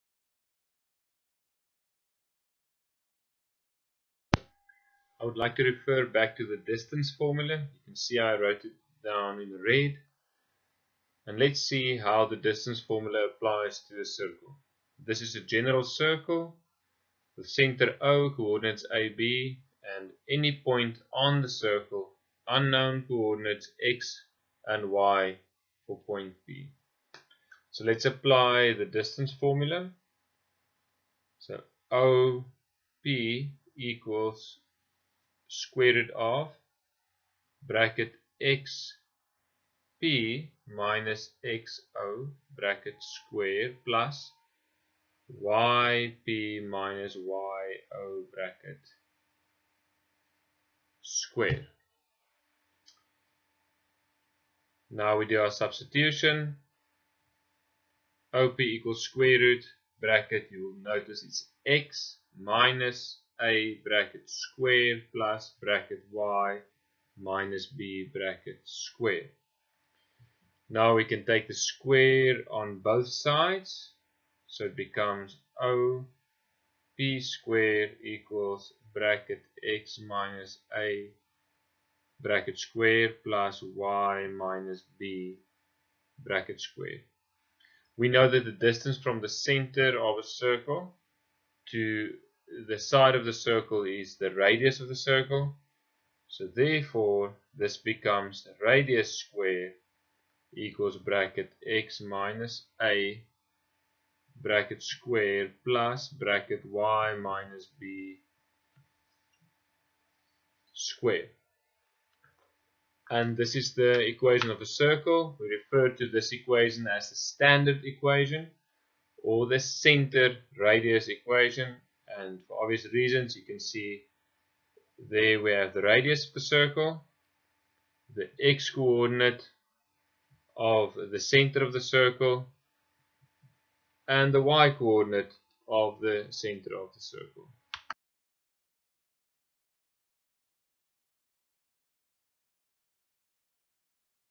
2e Equation of any circle
Grade 12 Mathematics Term 2

The video explains how to calculate/derive the equation of a circle by using the distance formula and how to get the coordinates of the centre of the circle and the radius.

Presented by Johan Oosthuizen
Global Leadership Academy